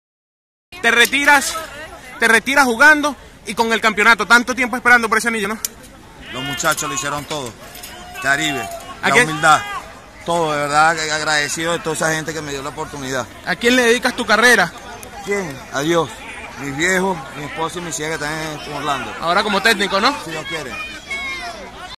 Tomás Pérez habla después del segundo título de Caribes
Anzoátegui 8-3 Magallanes
Estadio José Bernardo Pérez